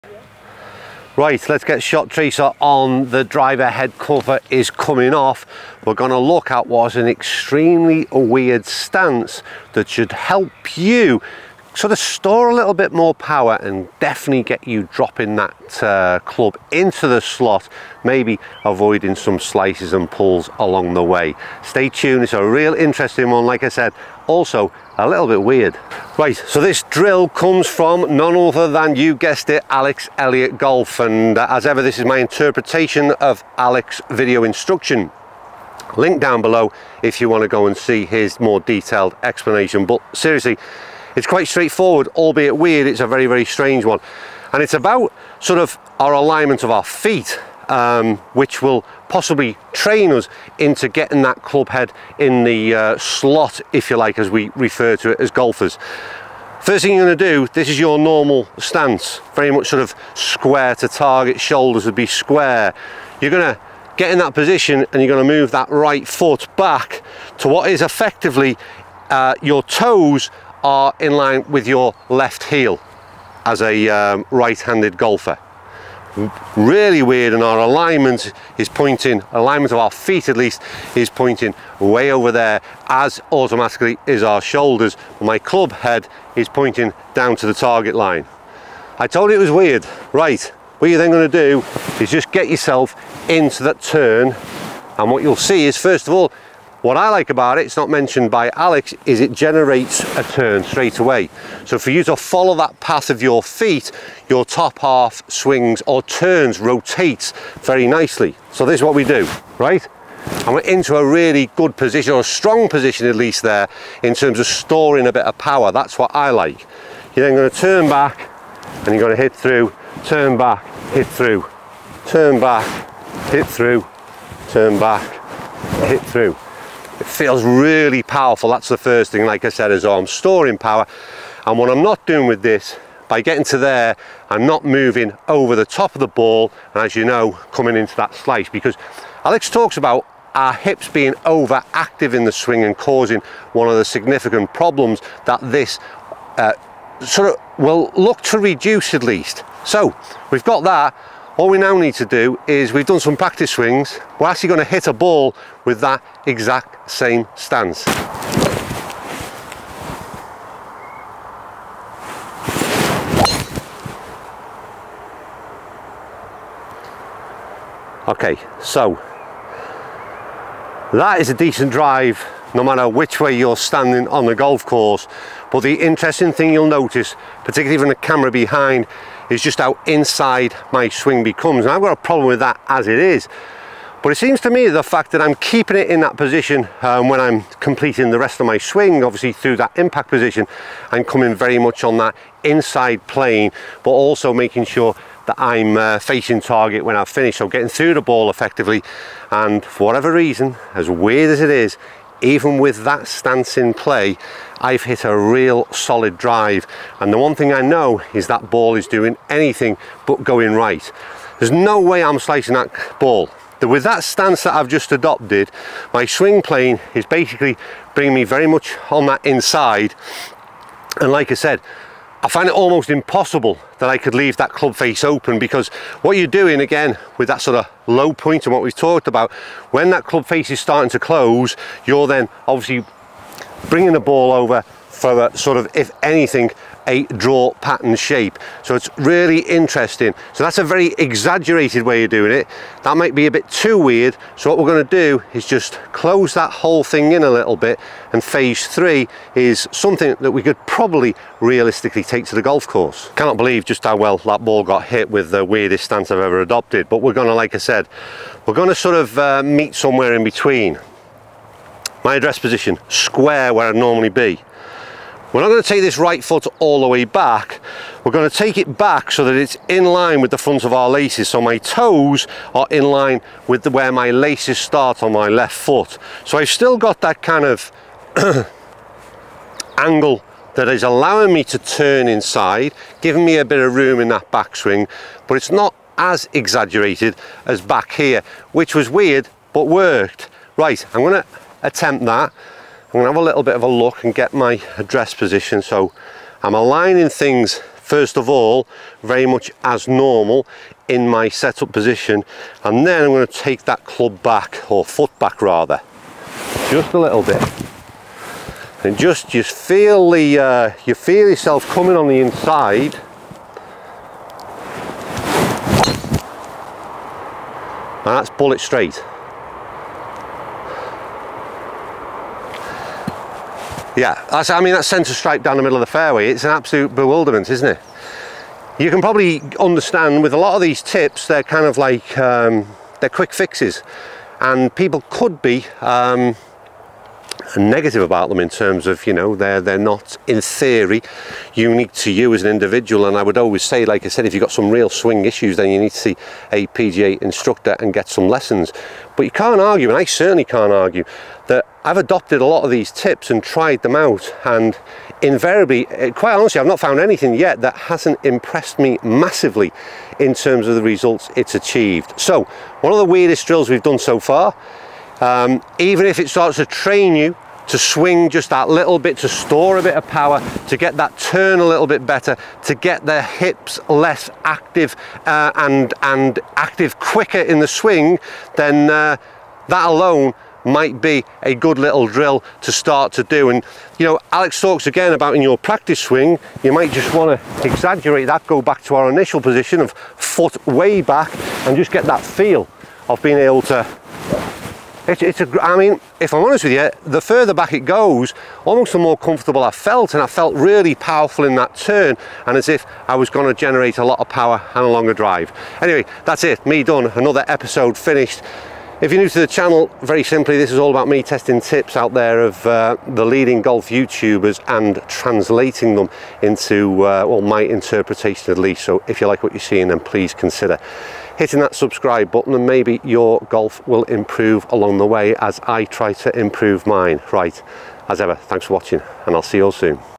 This WEIRD ADJUSTMENT will get you hitting driver from the INSIDE EVERY SINGLE TIME!
For all products featured in today's video and all your golf product requirements